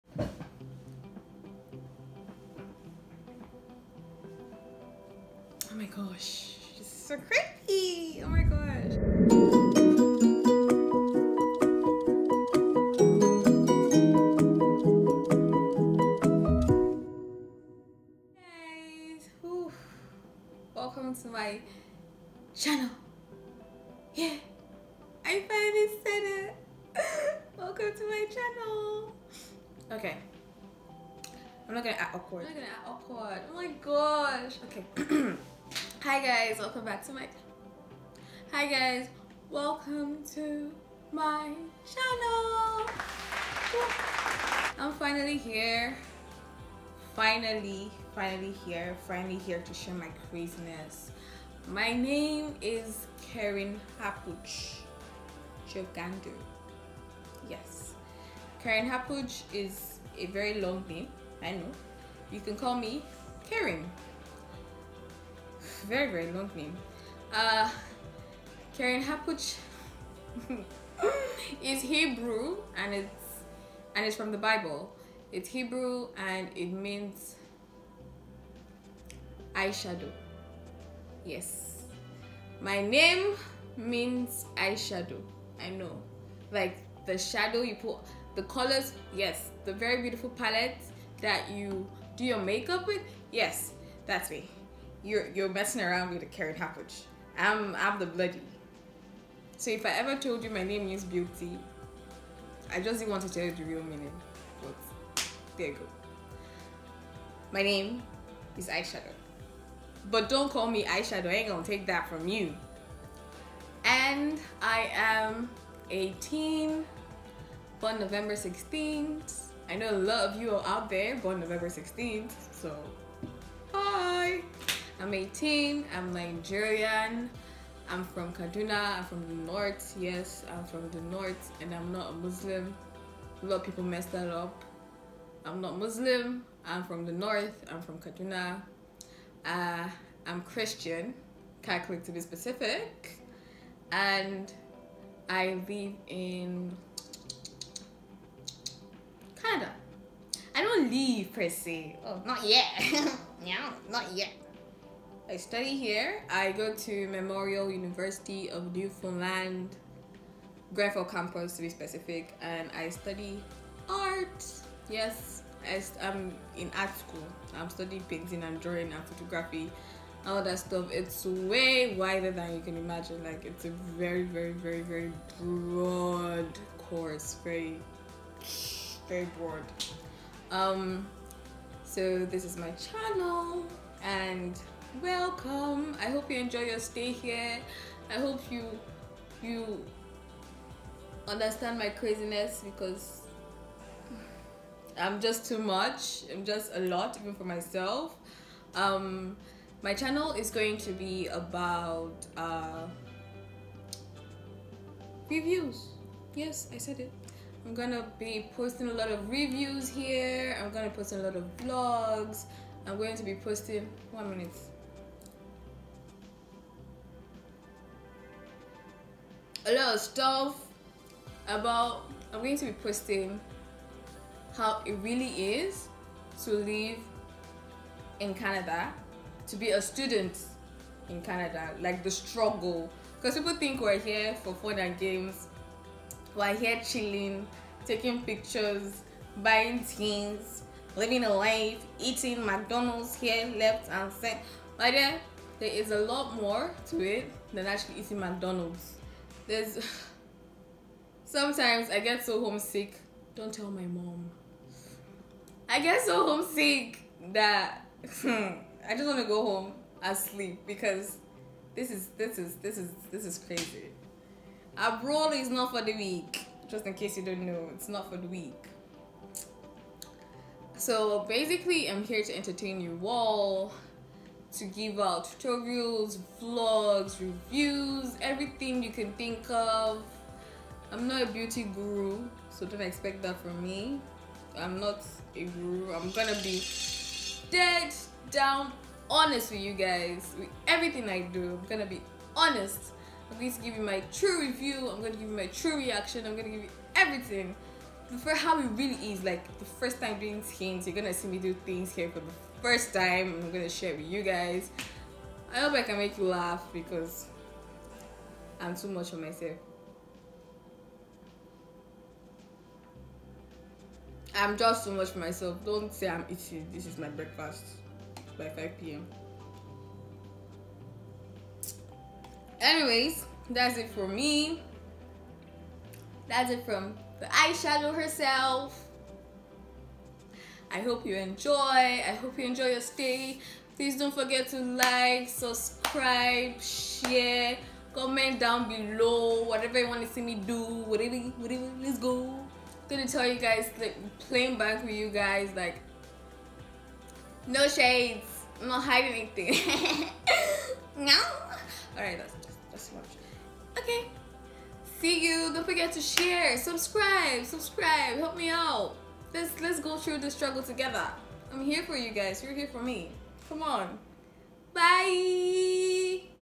GET TO KNOW ME TAG//NEW YOUTUBER
HI GUYS,
My name is Kerenhappuch but you can call me Kerenh. I'm a new youtuber from Nigeria studying in Canada. This video contains basic information about me and what this channel will contain. I'm going to be filling you guys with lifestyle, reviews, vlogs and how i suvive and live in Canada as a black international student. I basically want to entertain you! I hope we all have fun.

Thank you so much and ENJOY!!!